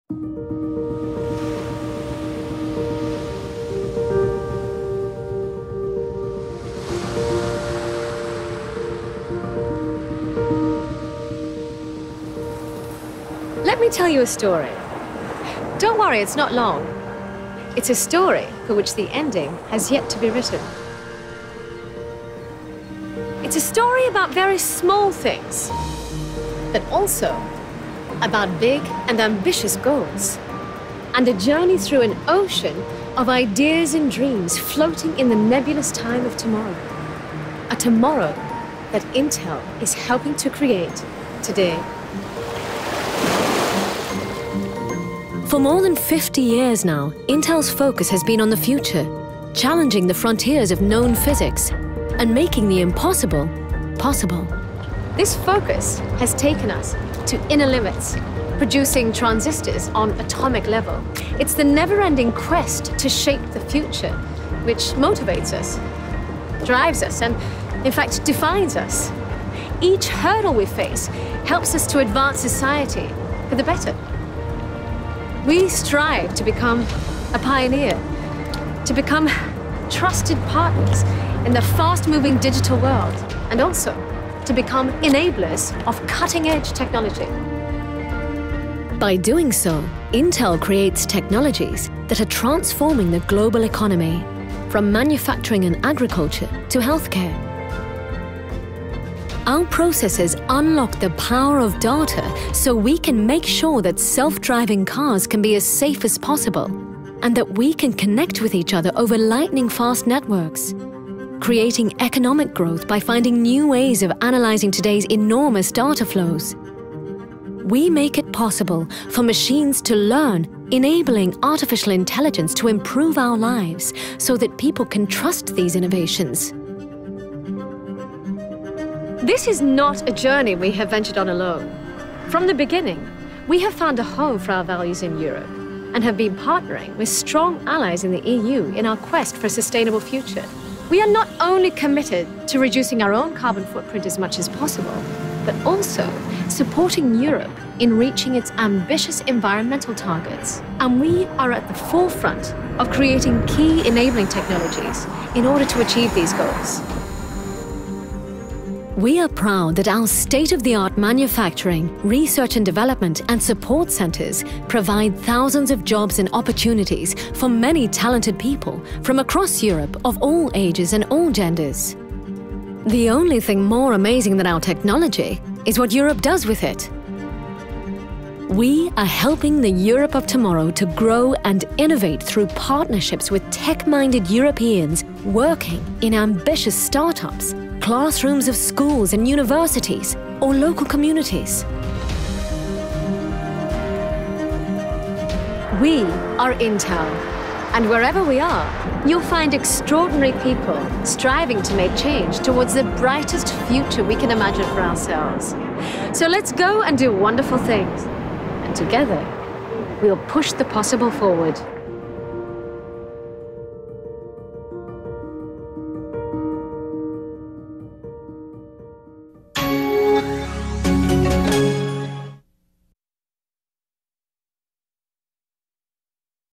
A Global Technology Leader in Europe | Intel Business
Present in Europe since 1970, Intel strives to become a trusted partner in a fast moving digital world. With one of the most advanced manufacturing site and partnerships with strong allies in Europe, Intel creates key enabling technologies (artificial intelligence, autonomous driving, IoT) able to shape the future and achieve ambitious environmental targets. Shorter version